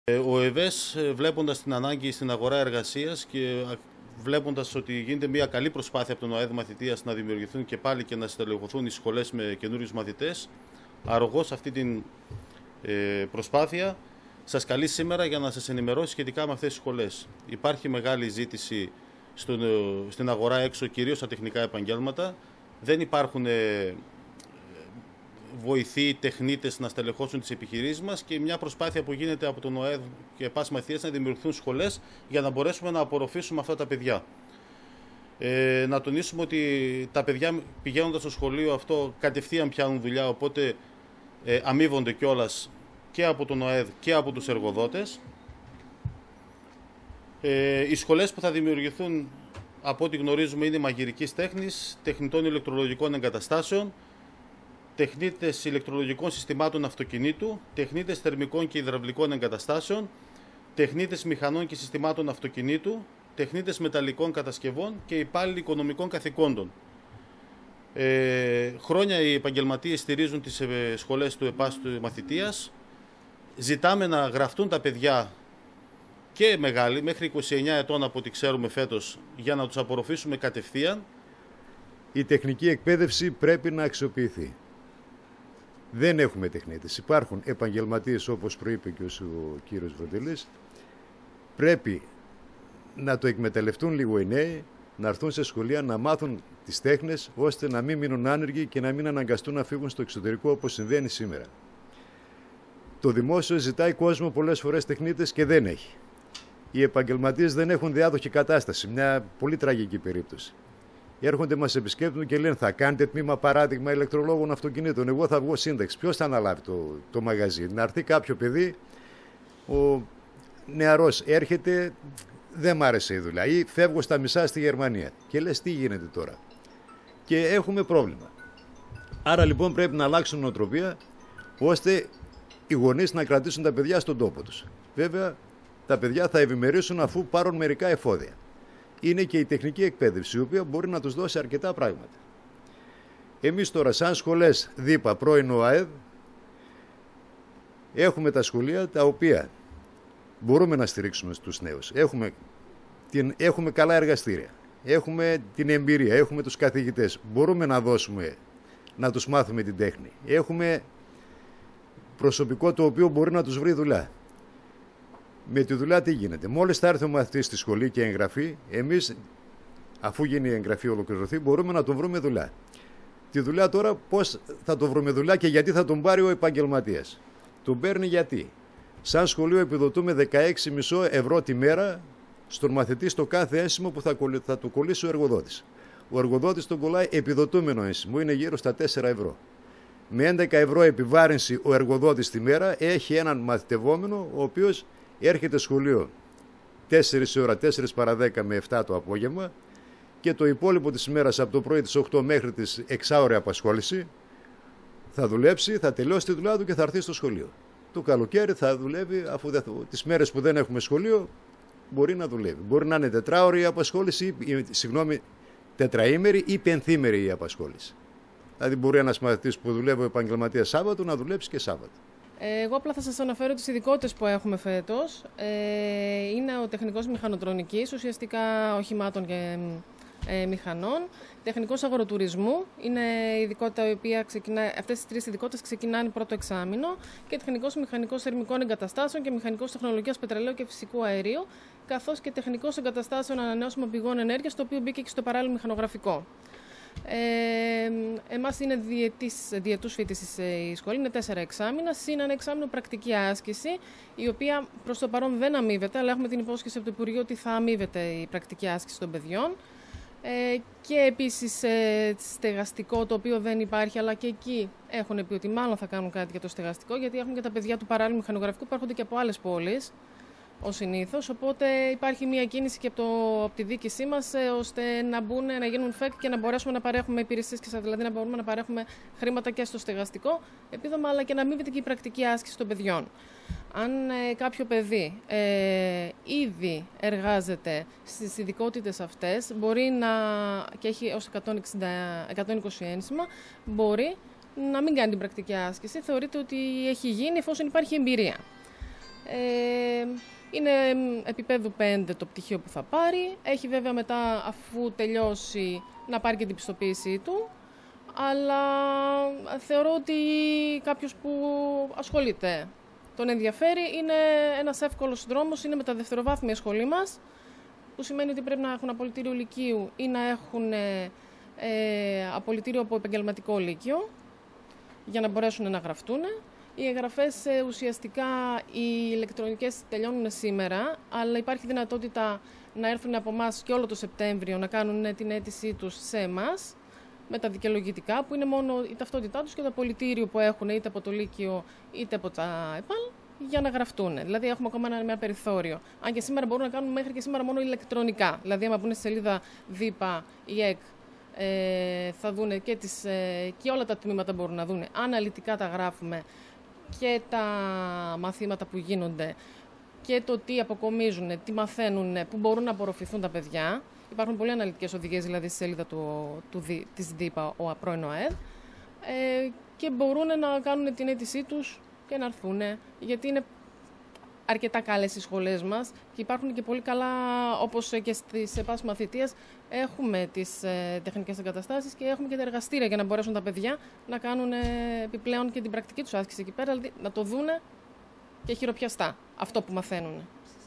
ΟΕΒΕΣΣ: Πρόσκληση για διάθεση θέσεων Μαθητείας Δ.ΥΠ.Α. 2022-2023
Ανοιχτό κάλεσμα για πρόσκληση συμμετοχής και διάθεση θέσεων Μαθητείας σε  μαθητευόμενους των ΕΠΑ.Σ της Δ.ΥΠ.Α. απηύθυναν μέσα από συνέντευξη τύπου που  παραχώρησαν στα τοπικά Μ.Μ.Ε. ο Γενικός Γραμματέας της ΟΕΒΕΣΣ, Δημήτριος Βροντελής,  και ο Διευθυντής της ΕΠΑ.Σ. Σερρών Παναγιώτης Αξιάρης.